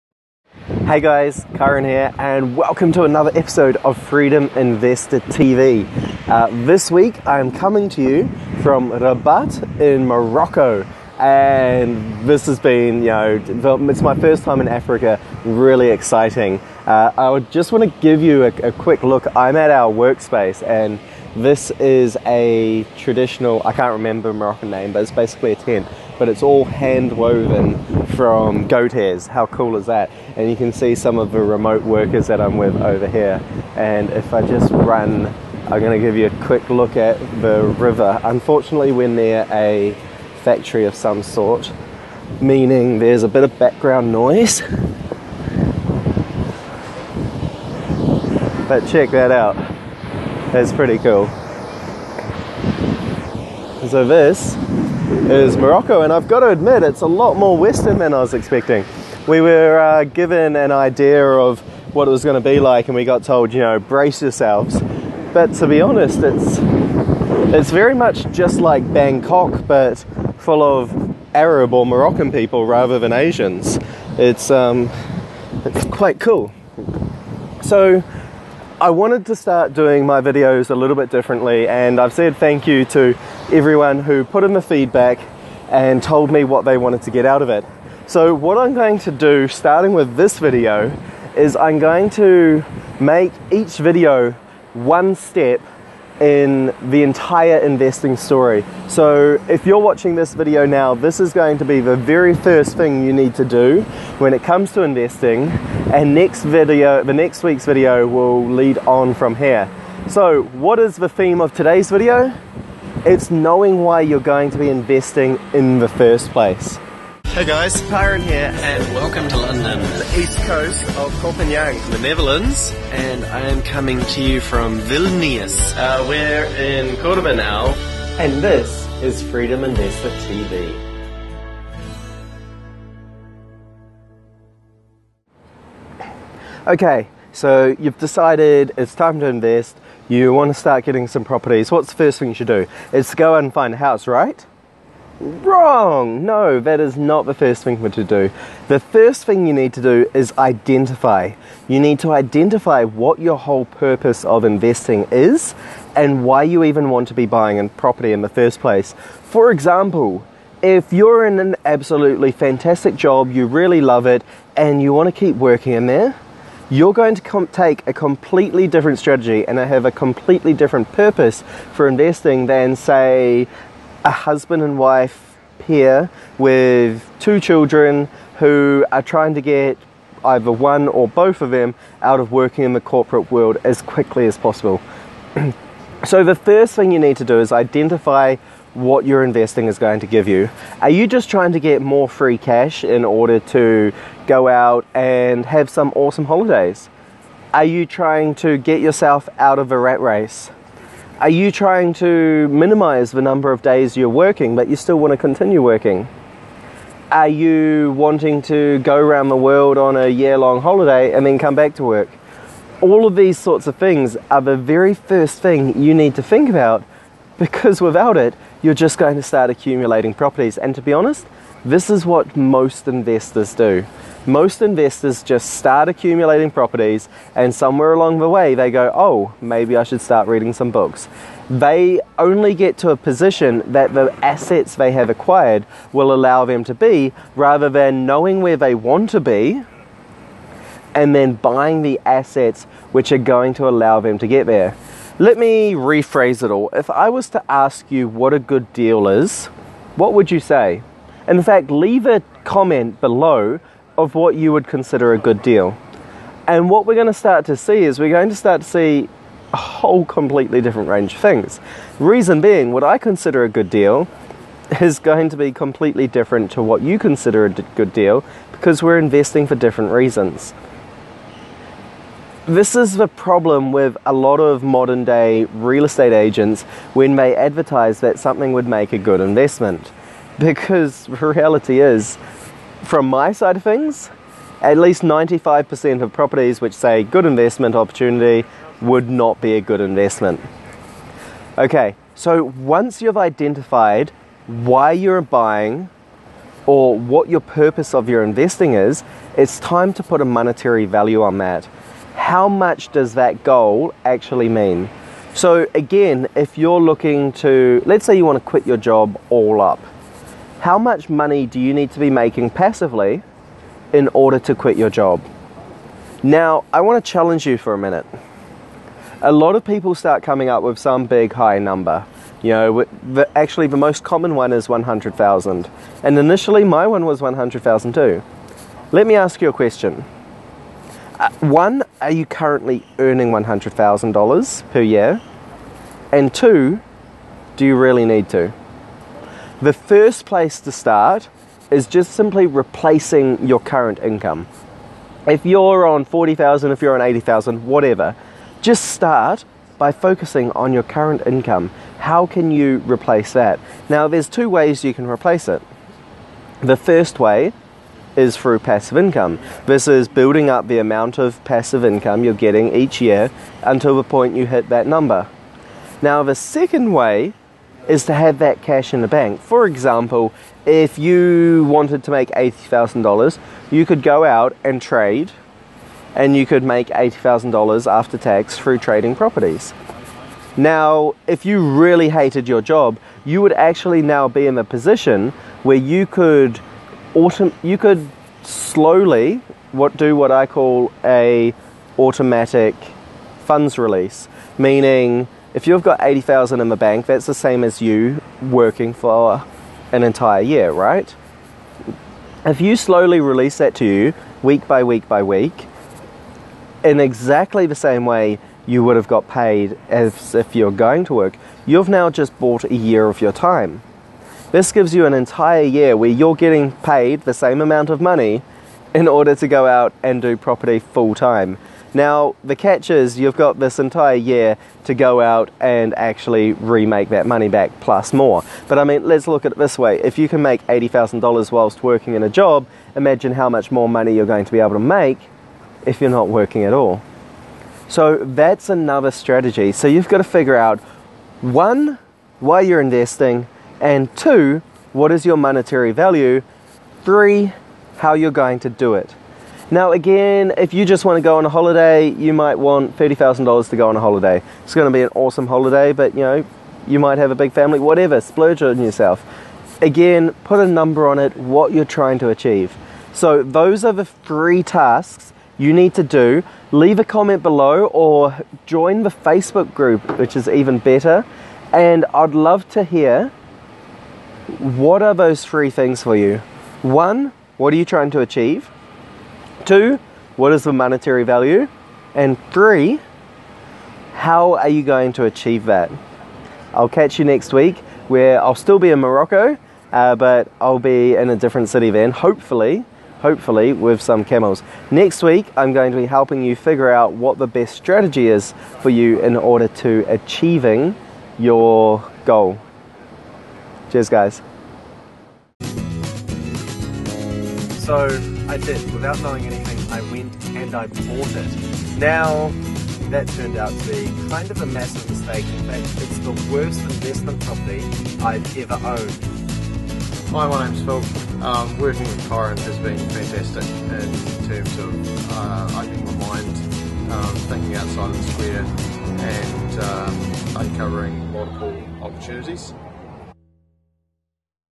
Why are you investing in the first place?
In the new style of Freedom Investor TV, we take a look at the first step you need to take when becoming a property investor... in fact this is probably the most important step out of all them - Figuring out why you even want to buy in the first place.